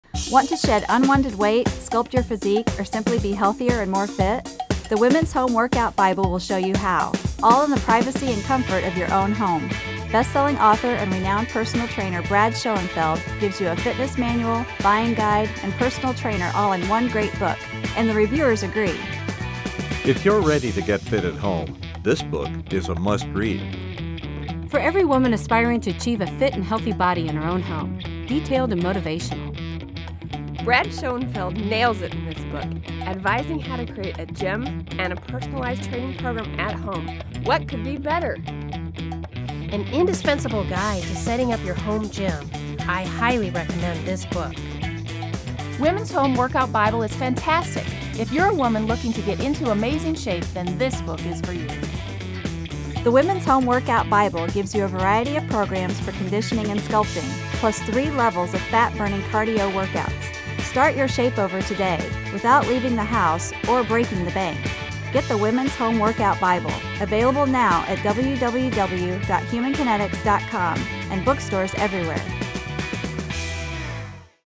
Women's Home Workout Bible
In "Women's Home Workout Bible", fitness expert Brad Schoenfeld makes sense of home-based workouts. The full-color book features 12 four-week programs for conditioning, sculpting, and core stability, plus three levels of fat-burning cardio workouts. It also has consumer tips for products and space guidelines for making the most of any home gym.